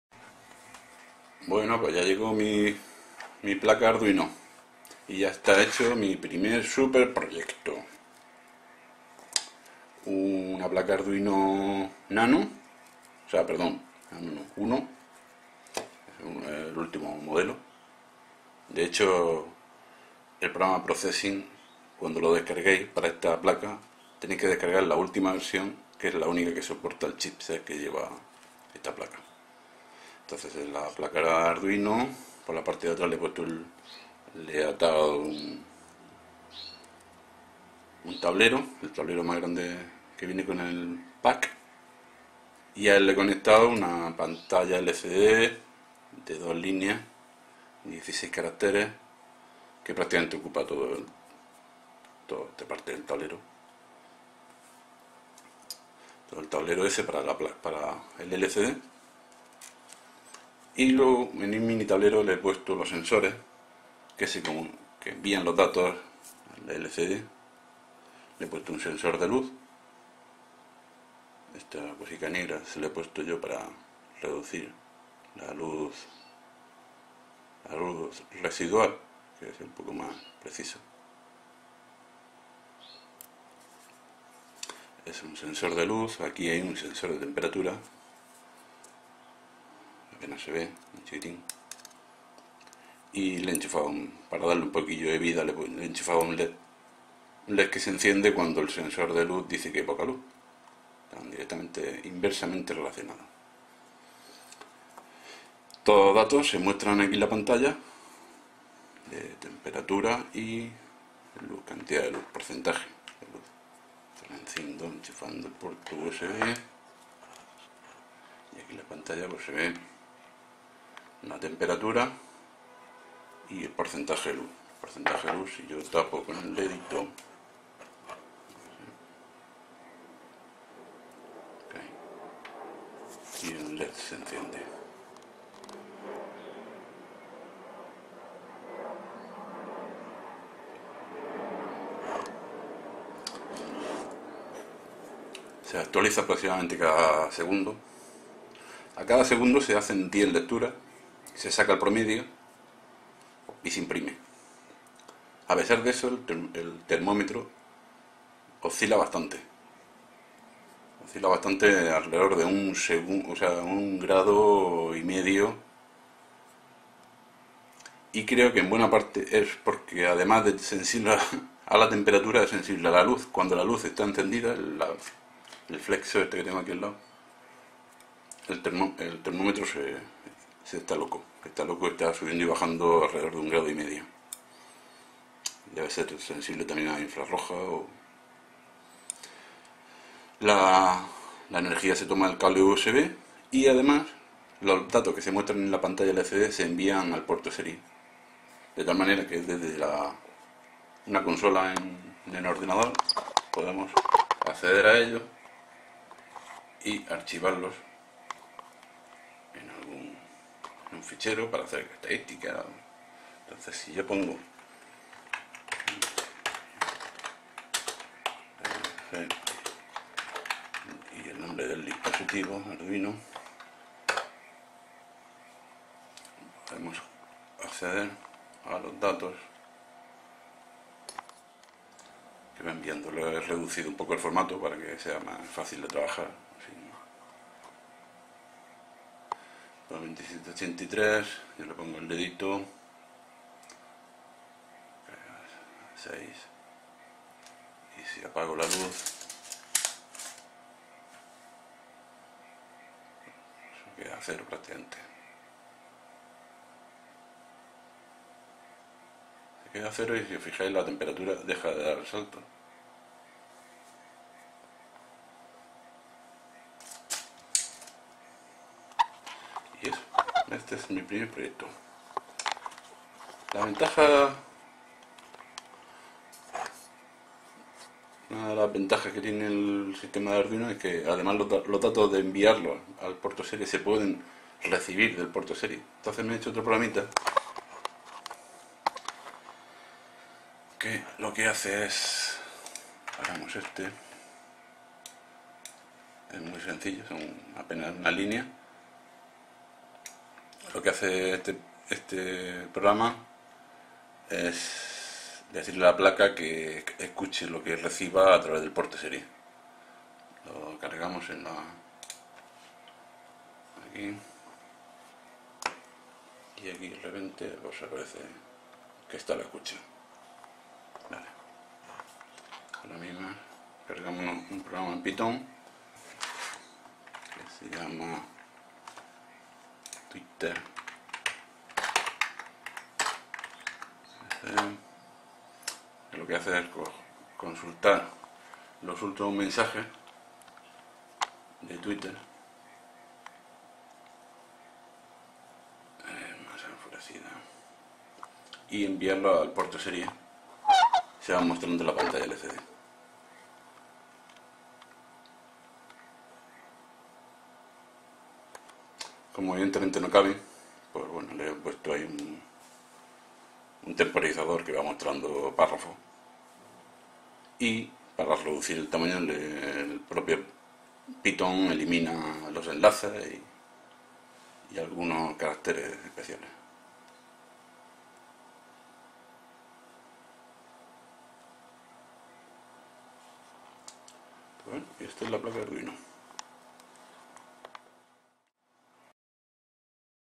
Arduino uno
Primer montaje con la placa Arduino recien estrenada